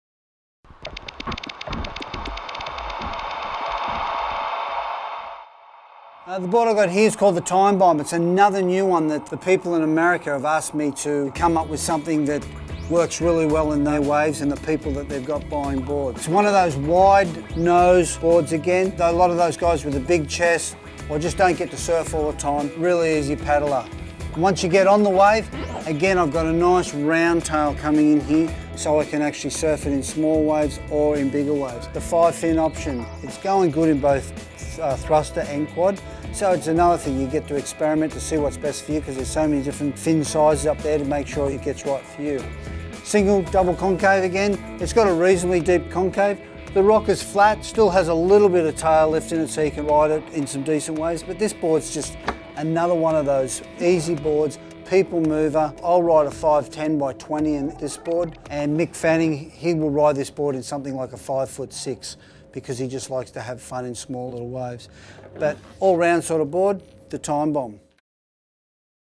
DHD Time Bomb Rip Curl Surfboards
Rip Curl shaper Darren Handley discusses the fundamentals of his board design The Time Bomb.